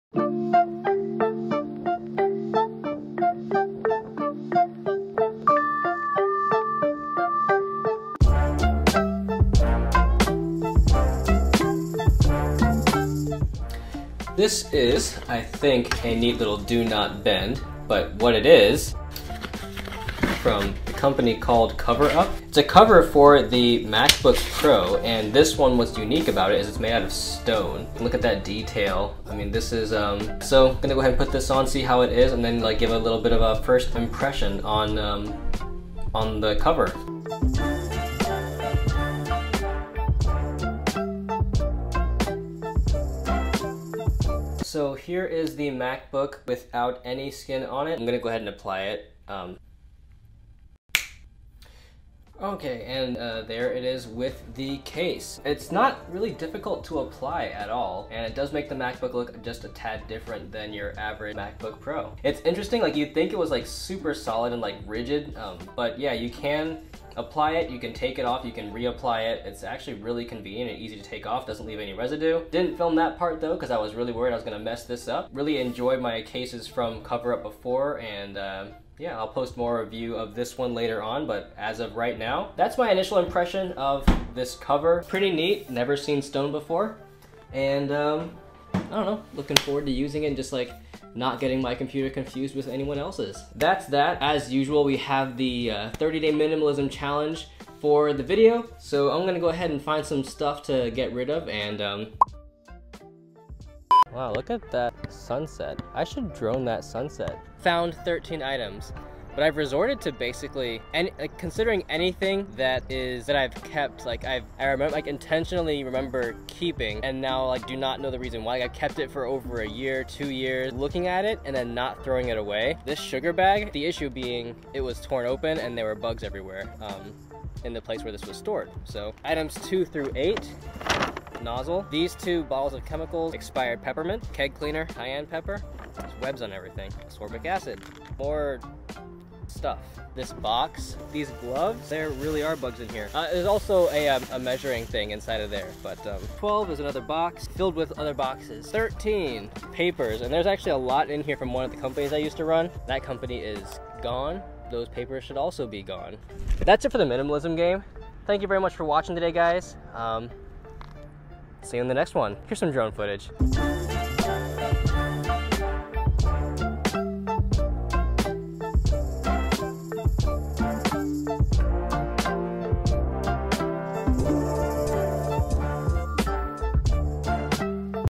Cover-Up Moonlight Stone Cover for MacBook Pro Impressions
Say what? There are stone covers for the MacBook Pro? Why yes, yes there are.

Also I threw away more stuff today for the MinsGame